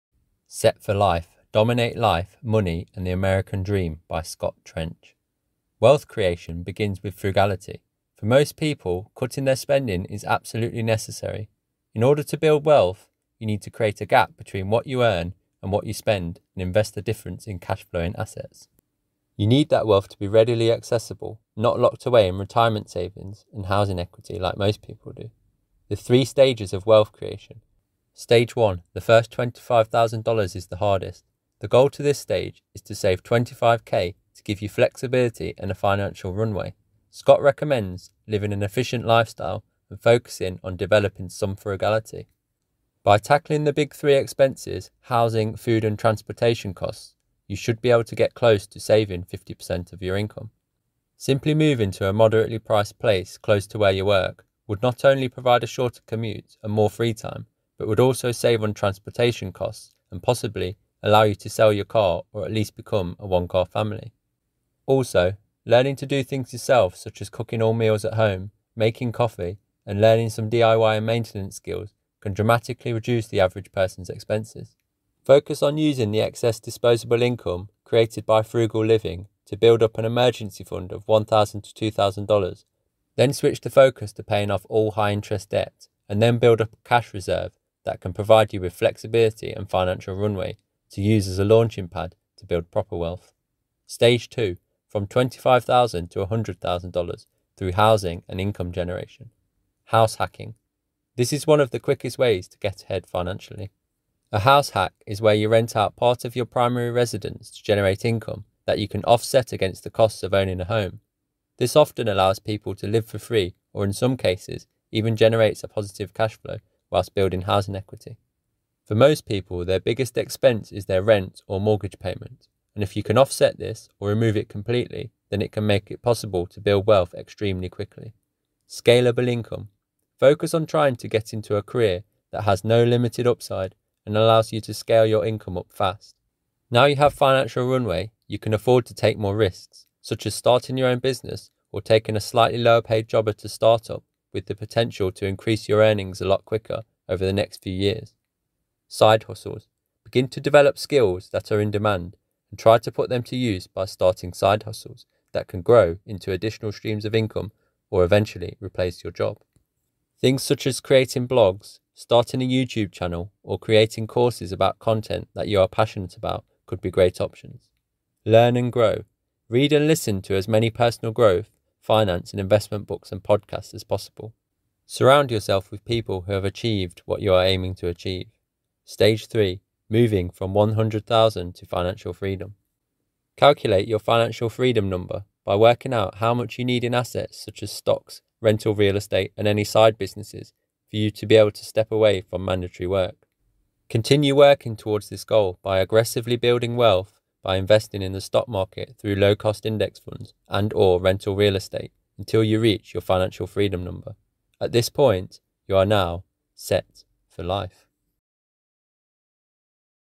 Set For Life By Scott Trench (Summary)
Book summary of Set For Life: Dominate Life, Money and the American Dream by Scott Trench.